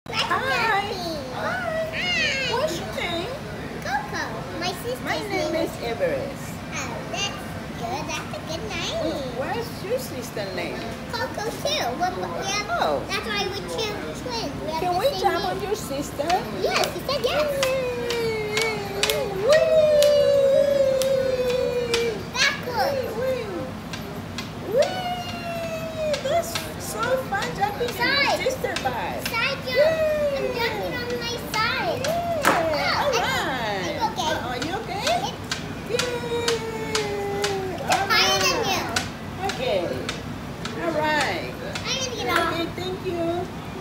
The Adventures of Emma & MIa
Emma and Mia start their adventure off with a slice of pizza and watermelon juice! Coco 1 and Everest enjoy jumping on Coco 2!!!